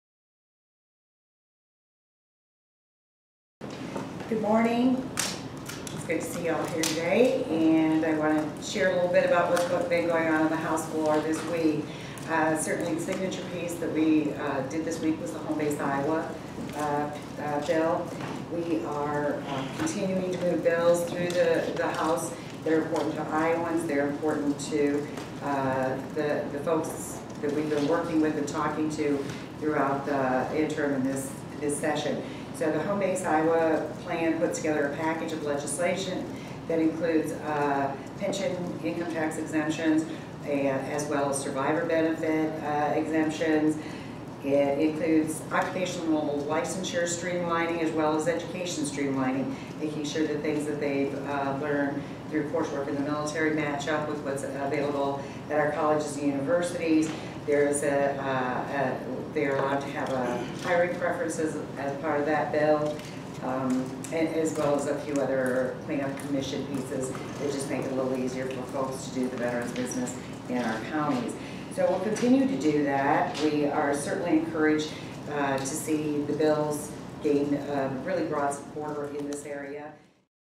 GOP Leader Upmeyer: House GOP Committed to Iowa Veterans
As we continue to move through the session, House Republicans continue to work on mainstream bills that are a priority for Iowans, including the Home Base Iowa plan.

This package of legislation is designed to attract veterans to Iowa and ensure they have the opportunities they need to be successful when they return.

Military Pension Income Tax Exemption:  Fully exempt military pensions from state income tax.

Survivor Benefits Income Tax Exemption:  Fully exempt military survivor benefits from state income tax.

Occupational Licensure:  Directs Iowa's occupational licensing boards to adopt rules allowing credit for military training and experience in the licensing process.

Hiring preference:  Allows private sector employers to grant a preference in hiring and promoting veterans.

Plate fees:  Eliminates the special plate issuance fees charged for plates associated with military service.

Monitor credits:  Requires community colleges and universities to file reports on the amount of credits they are giving veterans for their service in the military.

County Veterans Affairs:  Helps County Commissions of Veterans Affairs keep veterans records confidential as well as improving the policies dealing with the care and maintenance of veteran's graves.